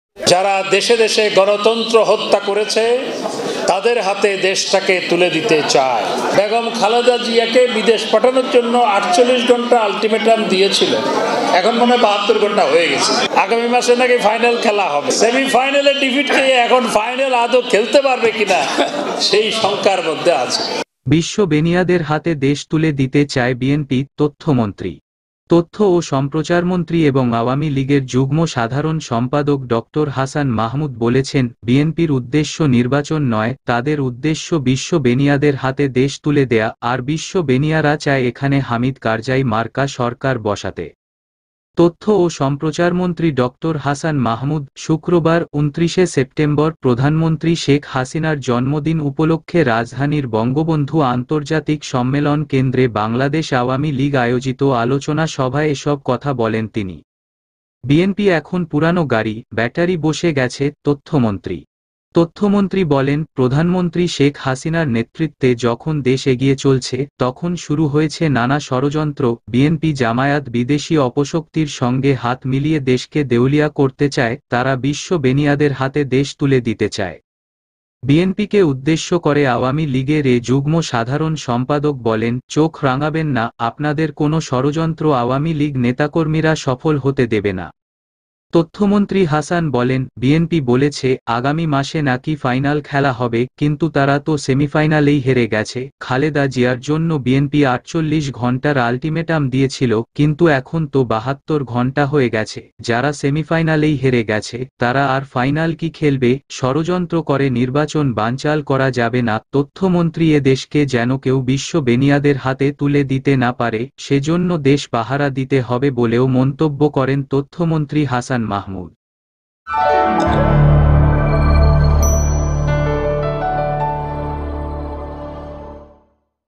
বিএনপি বিশ্ব বেনিয়াদের কাছে দেশকে তুলে দিতে চায় : তথ্য ও সম্প্রচারমন্ত্রী
বিশ্ব বেনিয়াদের হাতে দেশ তুলে দিতে চায় বিএনপি: তথ্যমন্ত্রী

তথ্য ও সম্প্রচারমন্ত্রী এবং আওয়ামী লীগের যুগ্ম সাধারণ সম্পাদক ডক্টর হাছান মাহমুদ বলেছেন, বিএনপির উদ্দেশ্য নির্বাচন নয়, তাদের উদ্দেশ্য বিশ্ব বেনিয়াদের হাতে দেশ তুলে দেয়া। আর বিশ্ব বেনিয়ারা চায় এখানে হামিদ কারজাই মার্কা সরকার বসাতে।

তথ্য ও সম্প্রচারমন্ত্রী ডক্টর হাছান মাহমুদ।

শুক্রবার (২৯ সেপ্টেম্বর) প্রধানমন্ত্রী শেখ হাসিনার জন্মদিন উপলক্ষে রাজধানীর বঙ্গবন্ধু আন্তর্জাতিক সম্মেলন কেন্দ্রে বাংলাদেশ আওয়ামী লীগ আয়োজিত আলোচনা সভায় এসব কথা বলেন